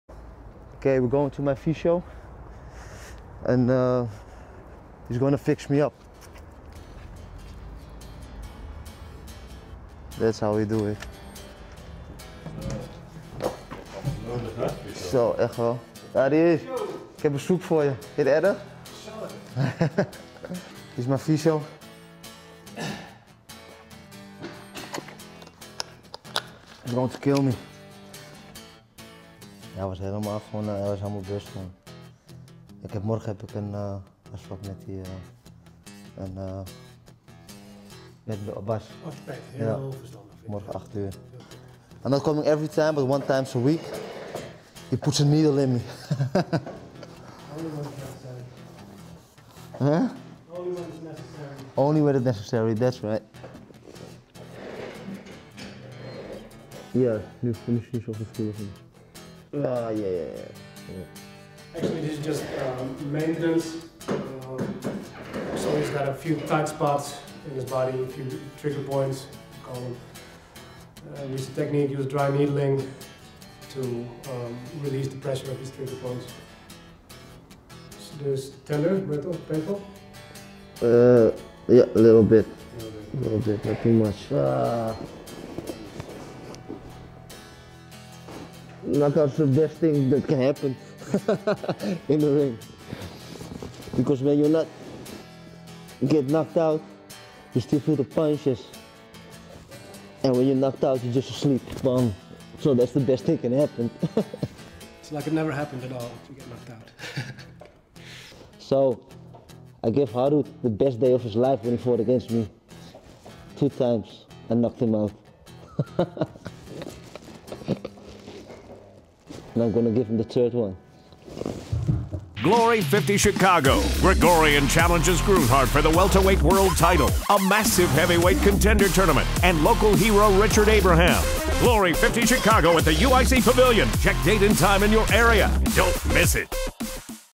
GLORY 50 Chicago: Murthel Groenhart Visits His Physical Therapist
The wear and tear of being a professional athlete can be grueling. Luckily sports therapy is readily available for all. Follow along as Murthel Groenhart visits his physical therapist in preparation for his first title defense.